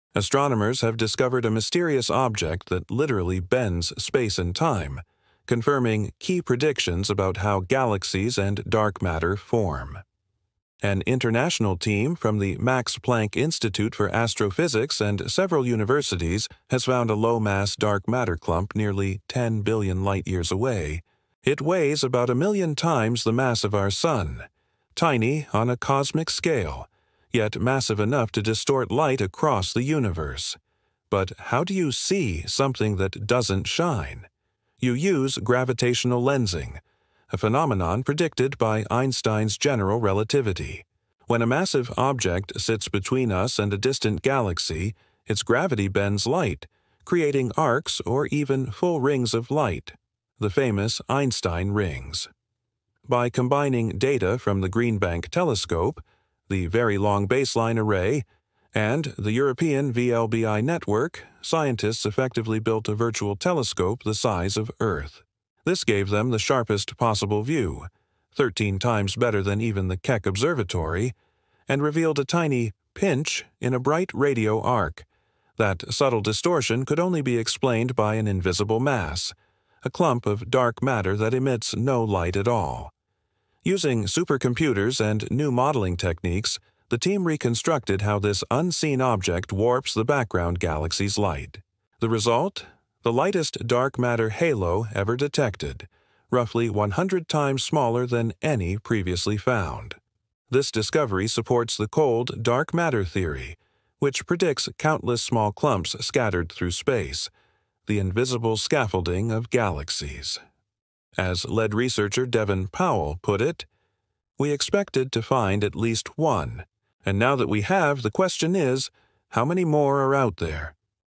Discovery of a low mass dark matter clump using gravitational lensing
“Astronomers Discover a Hidden Object That Warps Space and Time”

“A Mysterious Dark Matter Clump Found 10 Billion Light-Years Away”

“Einstein Was Right — and This Tiny Object Proves It”

“The Smallest Dark Matter Structure Ever Detected”